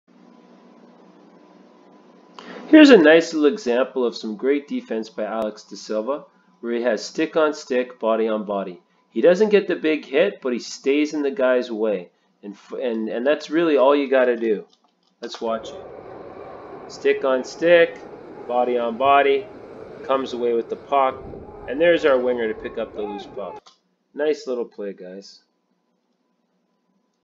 Good D stick on stick, body on body
You don't have to crush a guy on a 1 on 1. Just keep your body in front of them and have an active stick. Nice little play here.